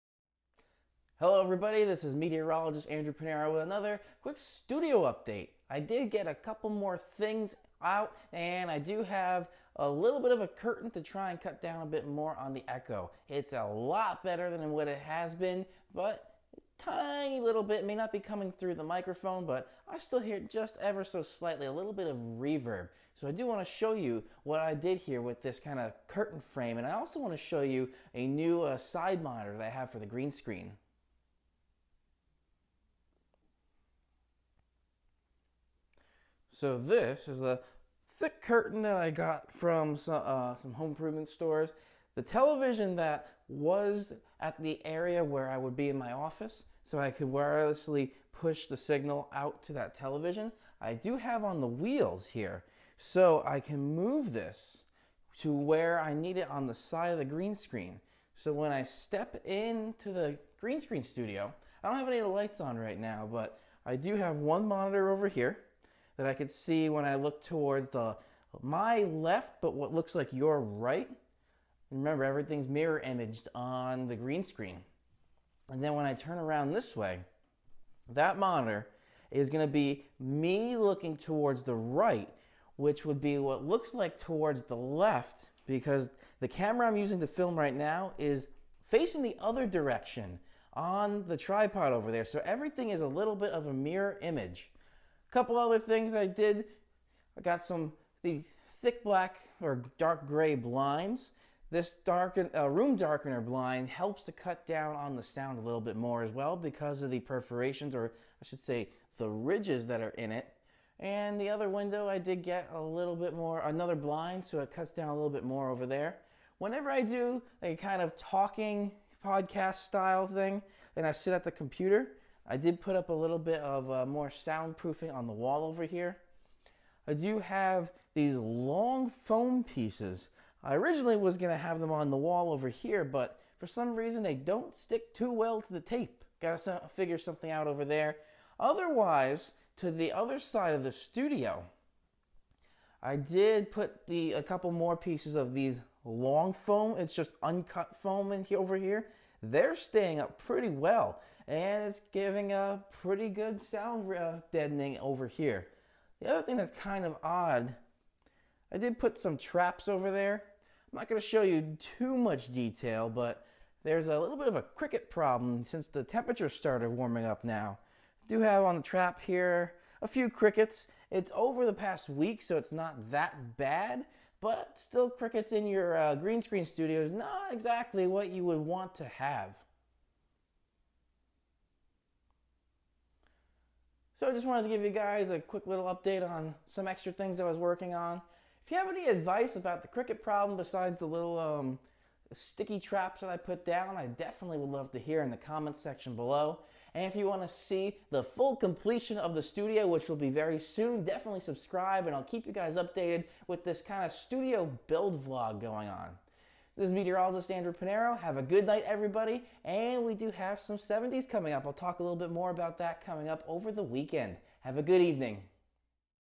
Studio Vlog Update 4/6/17
Sorry the video got publishes late, it was supposed to go up last night. I'm almost done with the studio! Just needs a bit more sound deadening and the latest thing, small cricket problem...